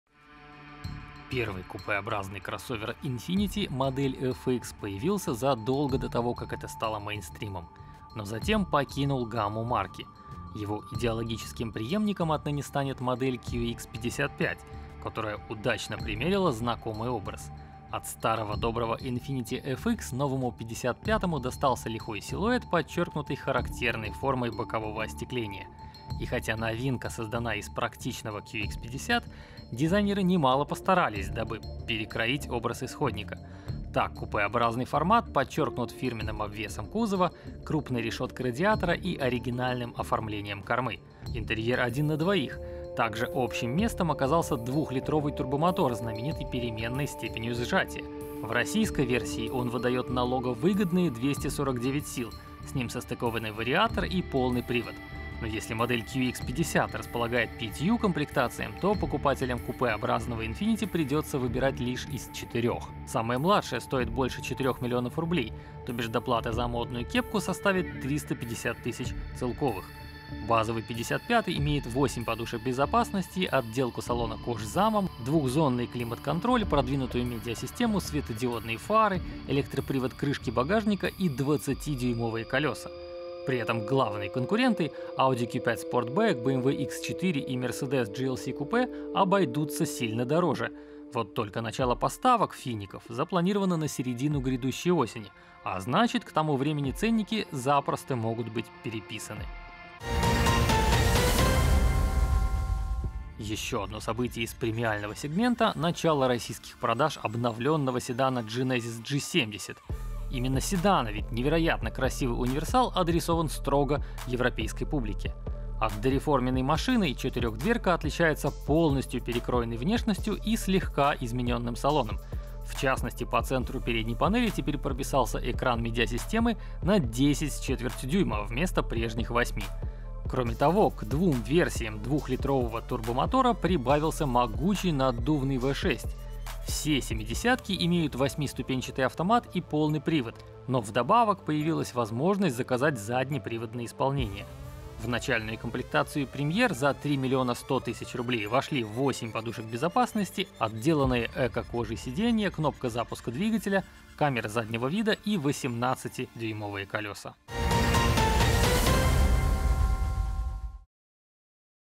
Infiniti QX55 согласился демпинговать. Genesis G70 обновился по всем фронтам | Новости с колёс №1594
Первый купеобразный кроссовер Infiniti — модель FX — появился задолго до того, как это стало мейнстримом. Но затем покинул гамму марки. Его идеологическим преемником отныне станет модель QX55, которая удачно примерила знакомый образ. От старого-доброго Infiniti FX новому «пятьдесят пятому» достался лихой силуэт, подчёркнутый характерной формой бокового остекления. И хотя новинка создана из практичного QX50, дизайнеры немало постарались, чтобы перекроить образ исходника. Так, купеобразный формат подчёркнут фирменным обвесом кузова, крупной решёткой радиатора и оригинальным оформлением кормы. Интерьер — один на двоих. Также общим местом оказался двухлитровый турбомотор, знаменитый переменной степенью сжатия. В российской версии он выдаёт налоговыгодные 249 сил. С ним состыкованы вариатор и полный привод. Но если модель QX50 располагает пятью комплектациями, то покупателям купеобразного Infiniti придётся выбирать лишь из четырёх. Самая младшая стоит больше четырёх миллионов рублей. То бишь доплата за модную «кепку» составит 350 тысяч «целковых». Базовый «пятьдесят пятый» имеет восемь подушек безопасности, отделку салона кожзамом, двухзонный климат-контроль, продвинутую медиасистему, светодиодные фары, электропривод крышки багажника и 20-дюймовые колёса. При этом главные конкуренты (Audi Q5 Sportback, BMW X4, Mercedes-Benz GLC Coupe) обойдутся сильно дороже… Вот только начало поставок «фиников» запланировано на середину грядущей осени, а значит, к тому моменту ценники запросто могут быть переписаны.

Ещё одно событие из премиального сегмента — начало российских продаж обновлённого седана Genesis G70. Именно седана, ведь невероятно красивый универсал адресован строго европейской публике… От дореформенной машины новинка отличается полностью перекроенной внешностью и слегка изменённым салоном: в частности, по центру передней панели теперь прописался экран медиасистемы на 10 с четвертью дюйма вместо прежних восьми. Кроме того, к двум версиям двухлитрового турбомотора прибавился могучий наддувный V6. Все «семидесятки» имеют 8-ступенчатый «автомат» и полный привод, но вдобавок появилась возможность заказать заднеприводные исполнения. В начальную комплектацию Premier за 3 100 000 рублей вошли восемь подушек безопасности, отделанные экокожей сиденья, кнопка запуска двигателя, камера заднего вида и 18-дюймовые колёса.